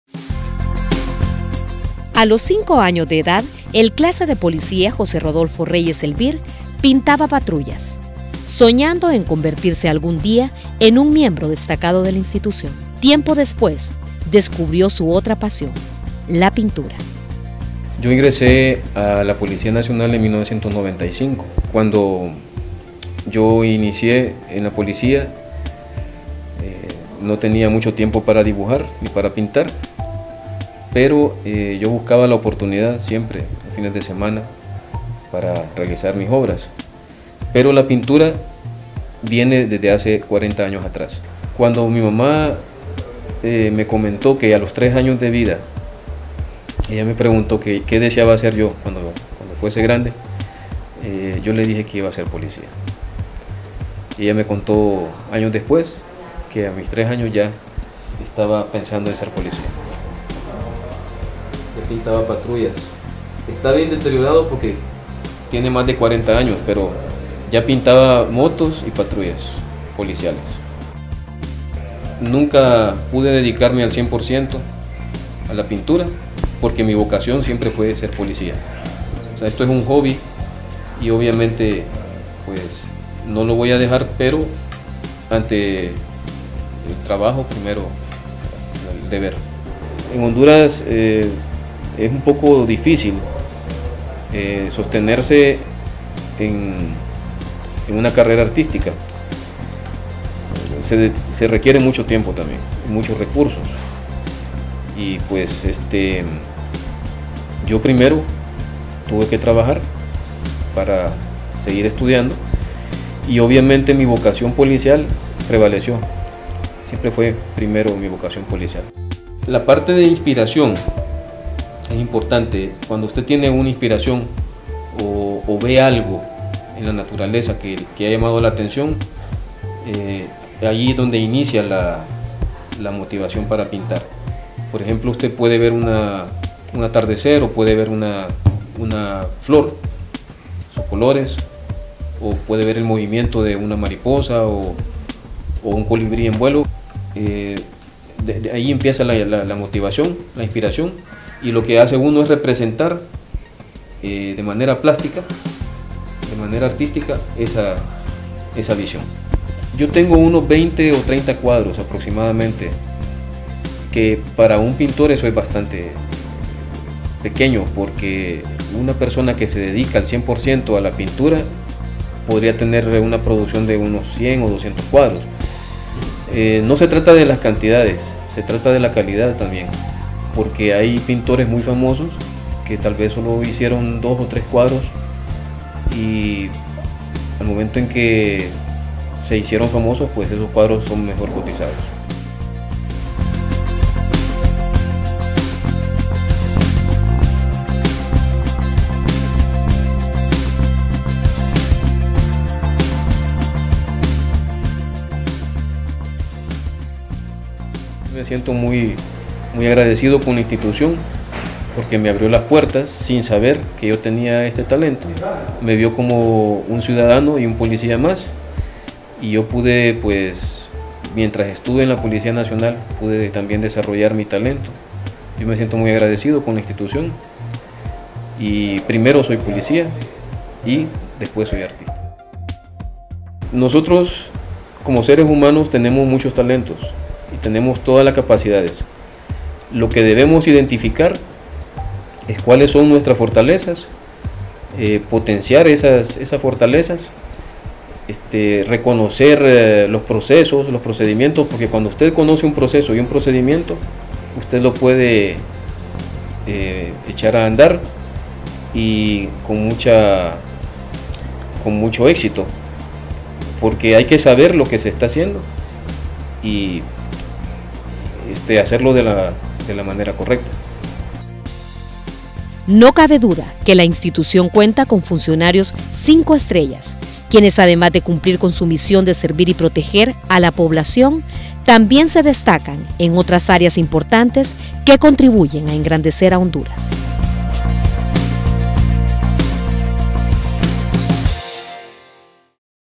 ¡Un sueño hecho realidad!
Conoce la historia del Clase I de Policía José Rodolfo Reyes Elvir